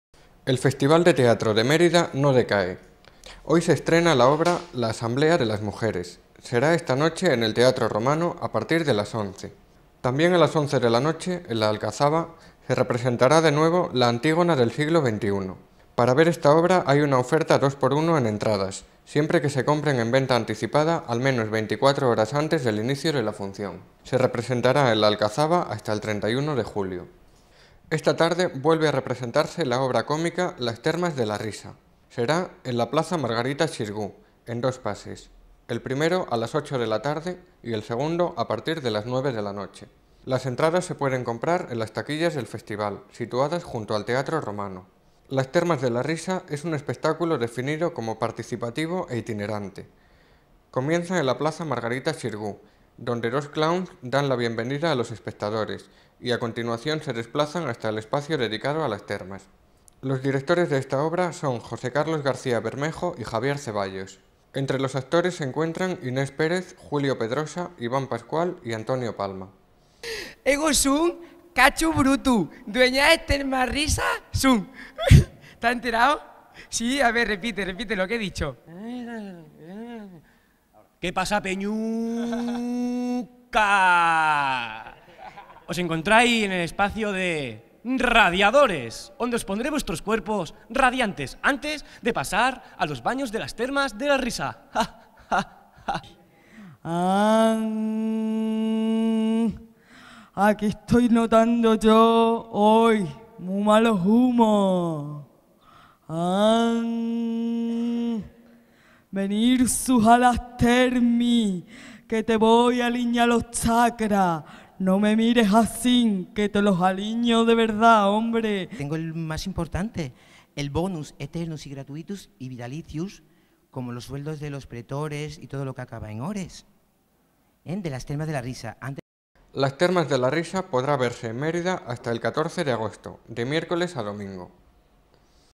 MÉRIDA 27/07/11 2x1 en entradas para el festival de Mérida
El Festival de Teatro de Mérida no decae.

Hoy es el estreno de la obra "La Asamblea de las Mujeres", que será esta noche en el Teatro Romano a partir de las 11 de la noche. También a las 11 de la noche, en la Alcazaba, se representará de nuevo "La Antígona del Siglo XXI".

Para ver esta obra hay una oferta 2x1 en entradas, siempre que se compren en venta anticipada al menos 24 horas antes del inicio de la función. Se representará en la Alcazaba hasta el 31 de julio.

Esta tarde vuelve a representarse la obra cómica "Las Termas de la Risa". Será en la Plaza Margarita Xirgú, en dos pases. El primero, a las 8 de la tarde, y el segundo, a partir de las 9 de la noche.

Las entradas se pueden comprar en las taquillas del Festival, situadas junto al Teatro Romano.

"Las Termas de la Risa" es un espectáculo definido como "participativo e itinerante". Comienza en la plaza Margarita Xirgú, donde dos clowns dan la bienvenida a los espectadores, y a continuación se desplazan hasta el espacio dedicado a las termas.

Los directores de esta obra son José Carlos García Bermejo y Javier Ceballos. Entre los actores se encuentran Inés Pérez, Julio Pedrosa, Iván Pascual y Antonio Palma.

"Las Termas de la Risa" podrá verse hasta el 14 de agosto, de miércoles a domingo.